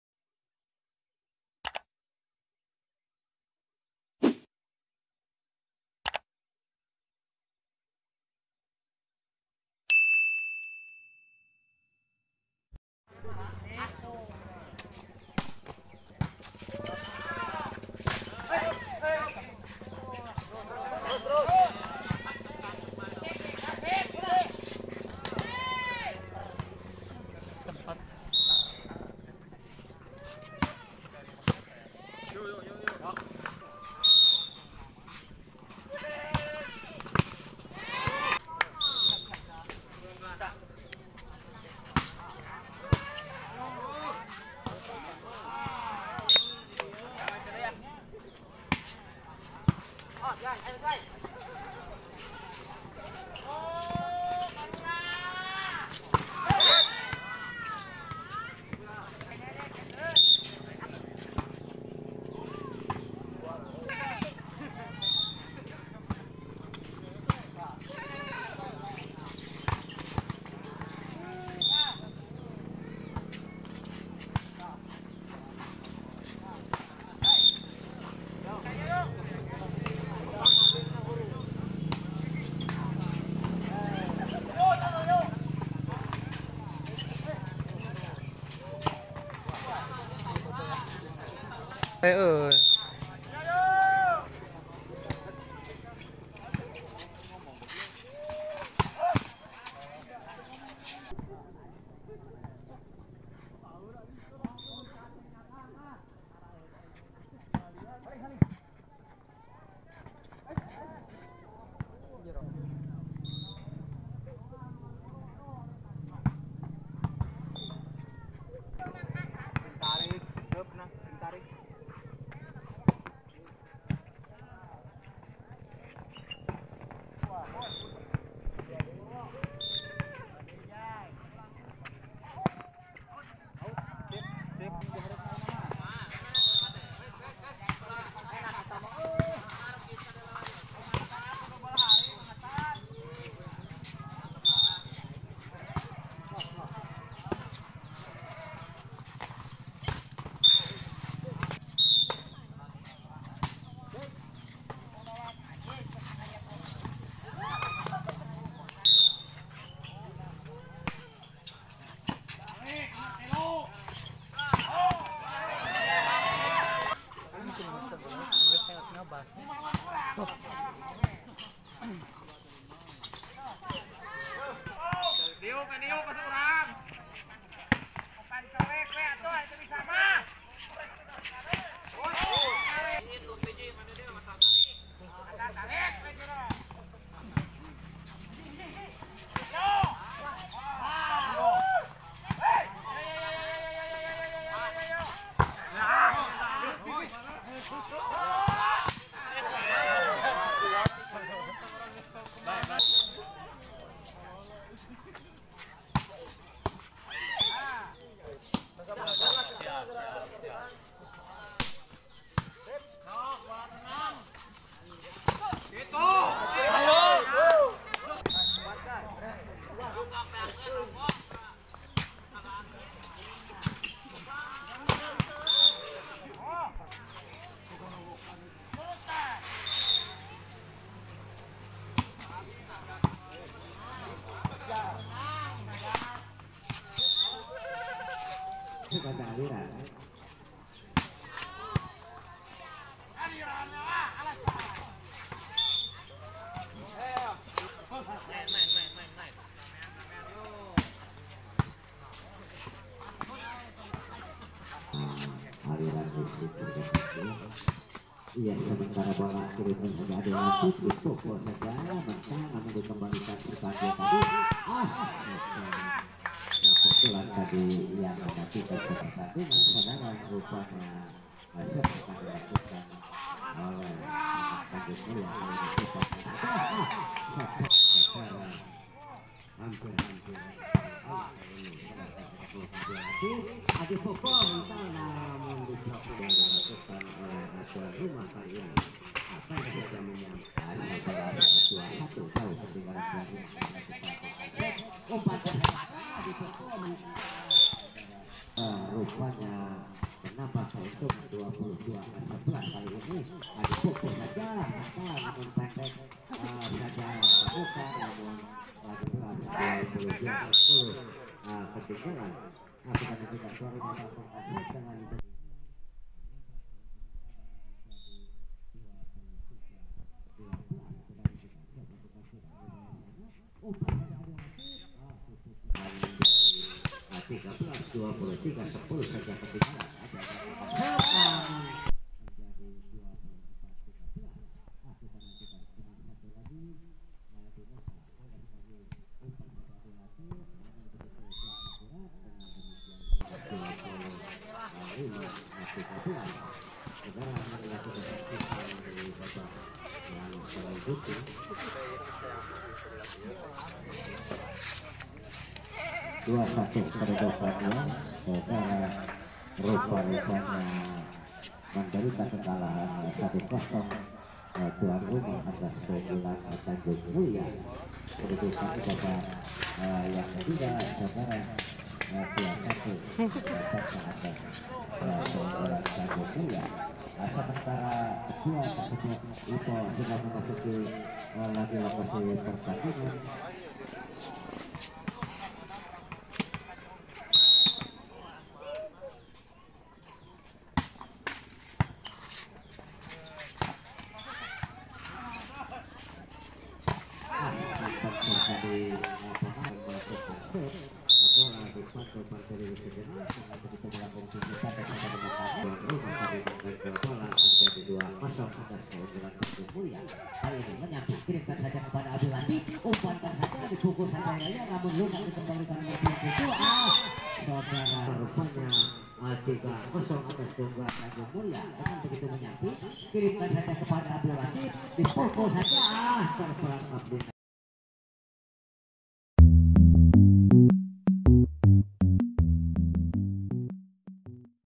SPIKE KERAS YUDI KENDIL LONCATAN SEDADA BIKIN PENONTON HISTERIS
Desa mekarmukti kec mekarmukti kab garut

Skj vs gibonet

Vidio Taken by : Rega Setia Agung
Vidio Editor : Rega Setia Agung